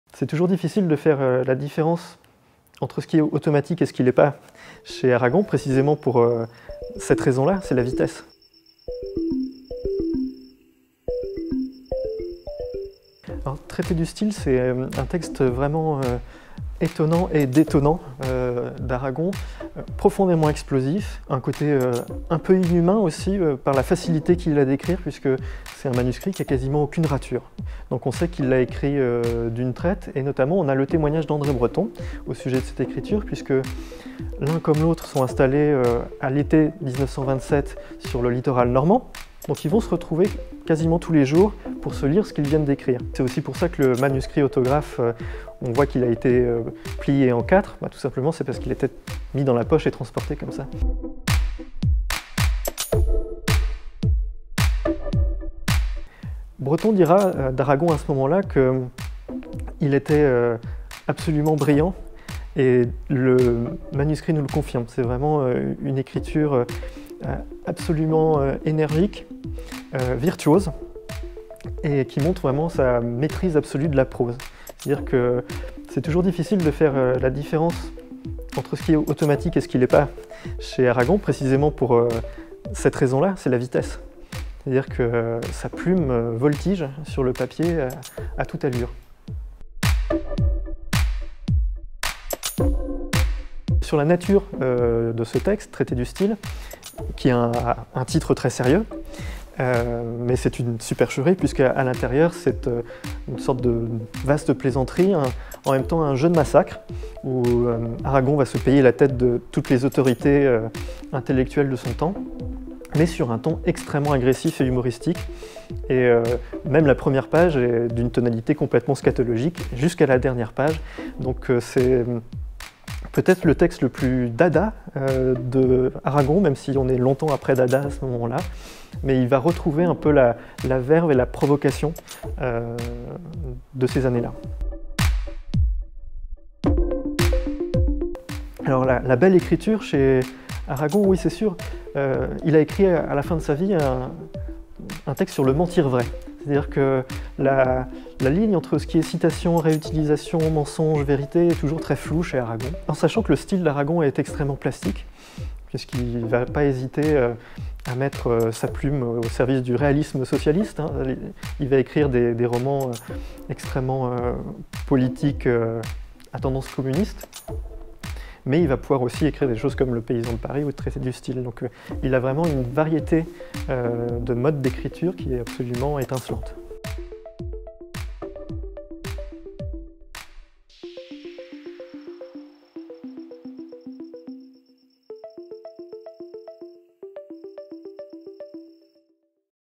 Traité du style de Louis Aragon
Avec la publication en 1928 de Traité du style, le poète surréaliste Louis Aragon révèle toute la virtuosité de son style, mise au service d’un texte polémique, empli d’humour et de provocation, dans la continuité de l’esprit Dada.

Olivier Wagner, conservateur et co-commissaire de l’exposition « L’Invention du surréalisme », présente ce texte emblématique du mouvement, dont le manuscrit est conservé à la BnF.

Du 19 mai au 14 août 2021 | François-Mitterrand

En savoir plus sur l'exposition « L’Invention du surréalisme : des Champs magnétiques à Nadja »